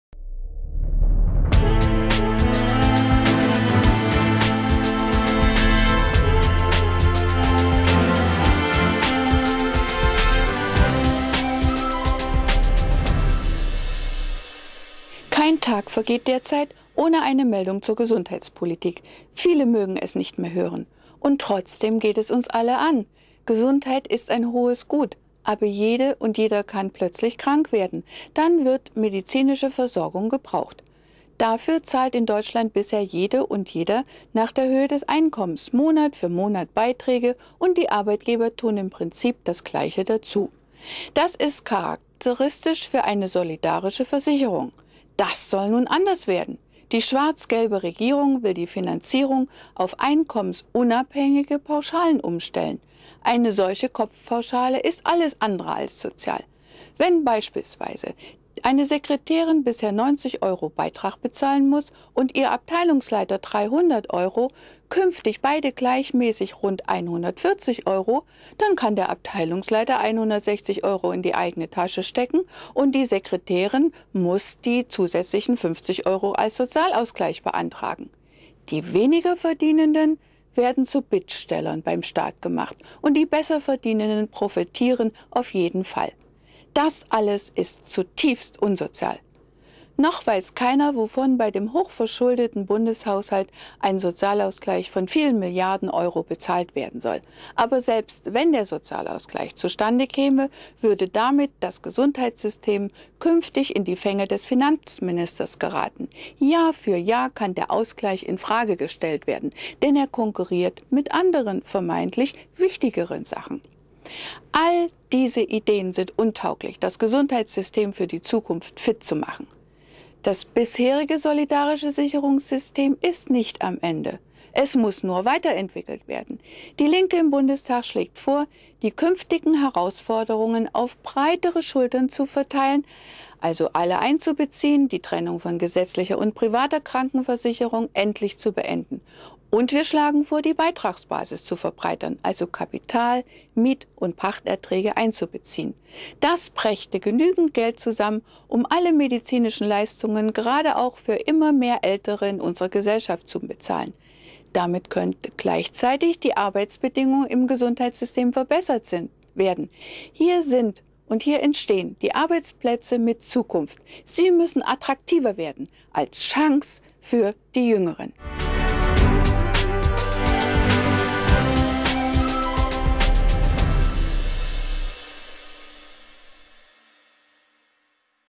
DIE LINKE, Martina Bunge: Gesundheit ist keine Ware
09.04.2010 - Für die medizinische Versorgung zahlt bisher in Deutschland jeder Beiträge nach der Höhe des Einkommens, und die Arbeitgeber tun im Prinzip das Gleiche dazu. Die schwarz-gelbe Bundesregierung will jetzt die Finanzierung dieser solidarischen Versicherung auf einkommensunabhängige Pauschalen umstellen. Diejenigen, die weniger verdienenden, werden zu Bittstellern beim Staat gemacht, und die Besserverdiener profitieren auf jeden Fall. Damit kann das Gesundheitssystem nicht fit für die Zukunft gemacht werden. Das bisherige solidarische Gesundheitssystem muss weiterentwickelt werden. DIE LINKE schlägt vor, künftig alle einzubeziehen, die Trennung von gesetzlicher und privater Krankenversicherung zu beenden und die Beitragsbasis zu verbreitern.